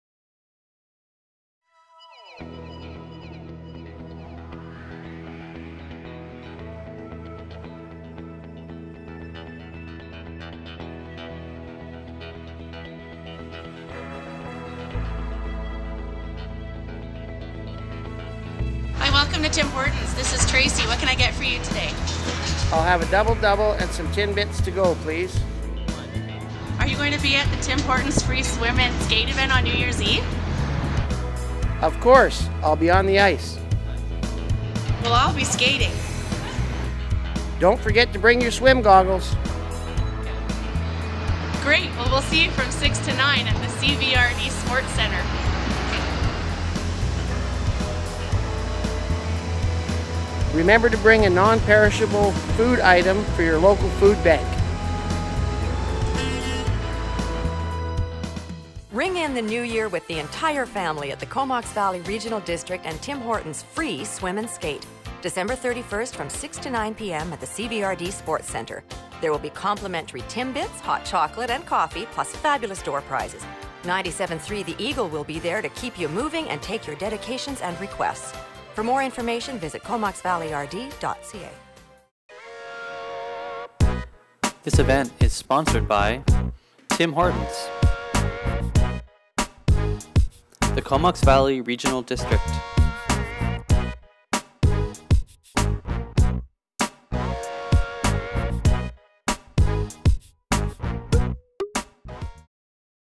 Not your usual drive-thru customer!
Comox Valley Regional District and Tim Hortons presents "Not your usual drive-thru customer" in promotion of the New Year's Eve swim and skate at the CVRD sports centre (3001 Vanier Road, Courtenay BC) from 6-9 p.m.